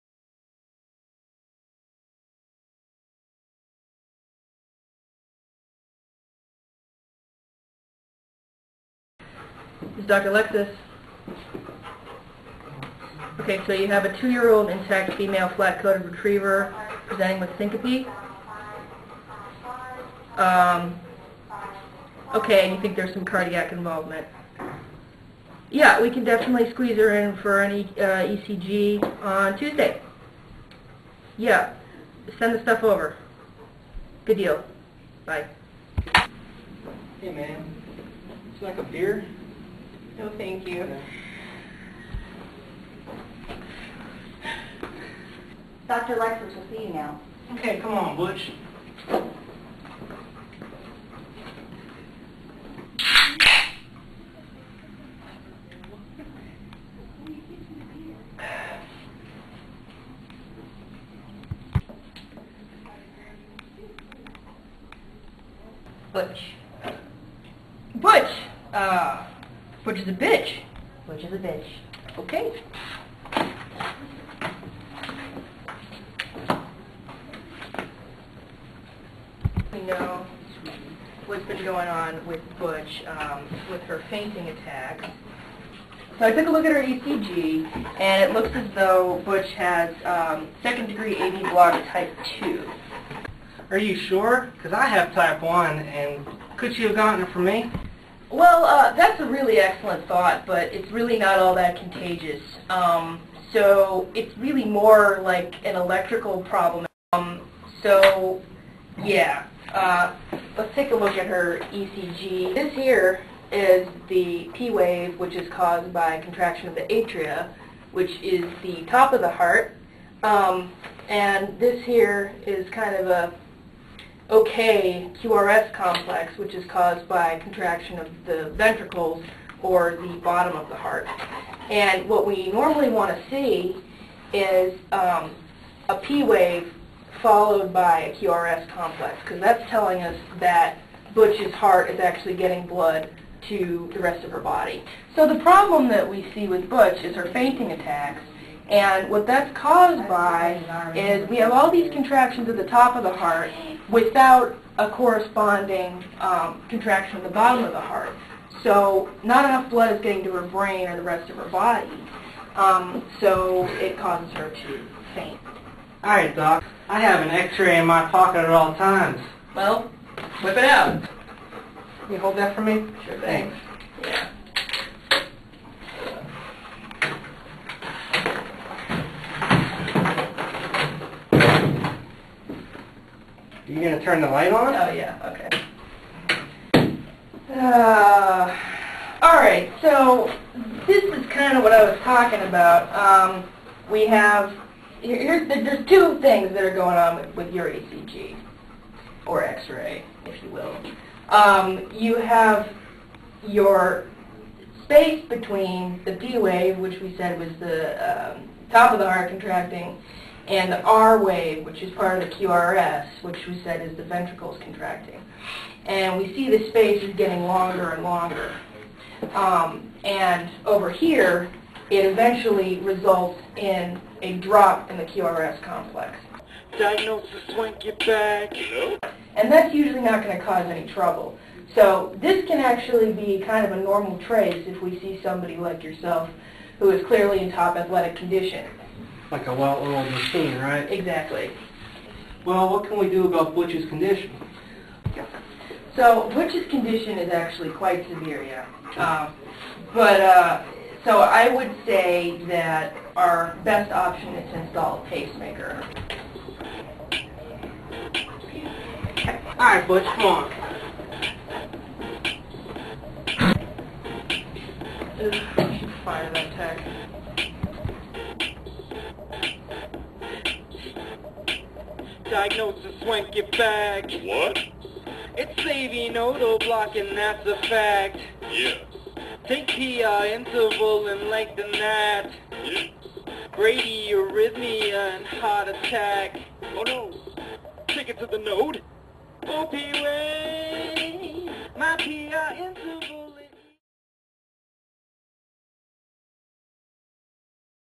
2nd Degree AV Block Featuring Maia the Flat-Coated Retriever
This video was produced as an assignment for our first year Physiology course at the University of Tennessee College of Veterinary Medicine.